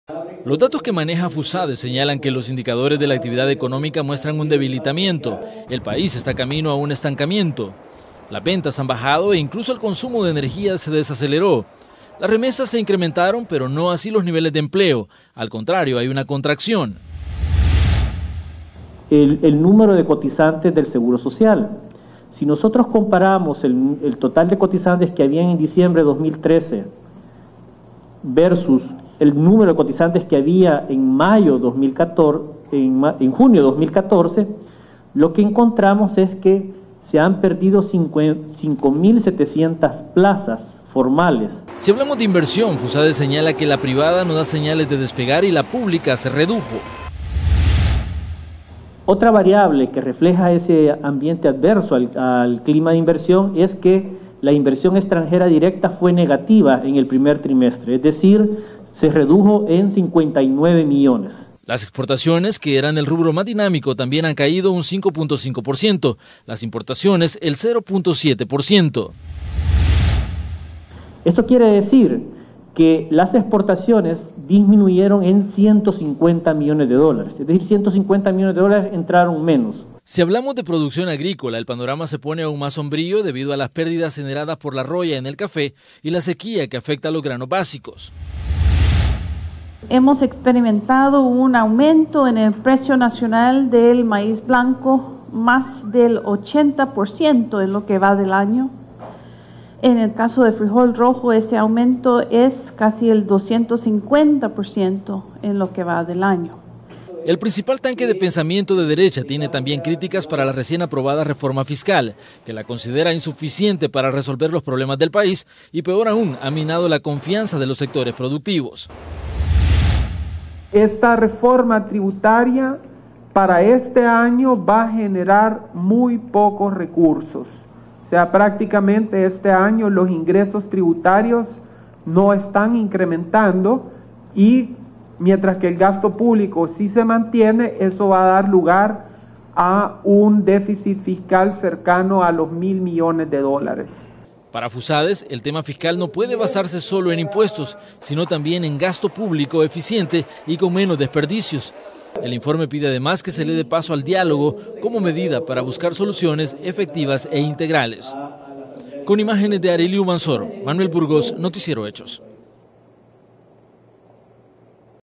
FUSADES INFORME DE COYUNTURA
Datos de Fusades señalan que los indicadores de la actividad económica muestran un debilitamiento, el país esta camino a un estancamiento. La inversión privada no da señales de despegar y la pública se redujo.